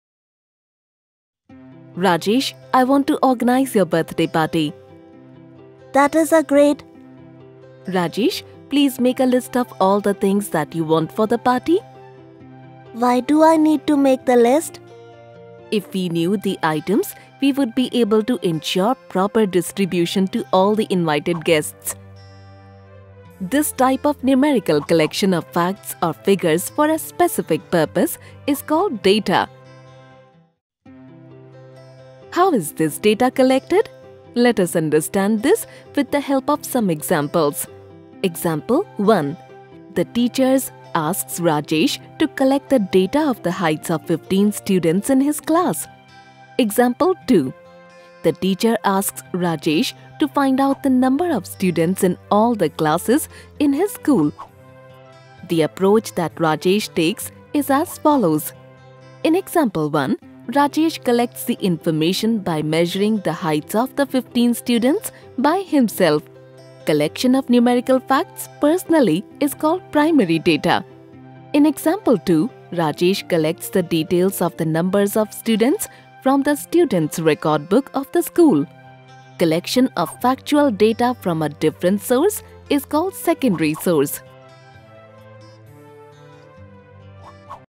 What is Data? Primary Data and Secondary Data [Explained with Example]
This video explains the following:
 - What is Data?
 - Primary Data
 - Secondary Data
with two real-life examples.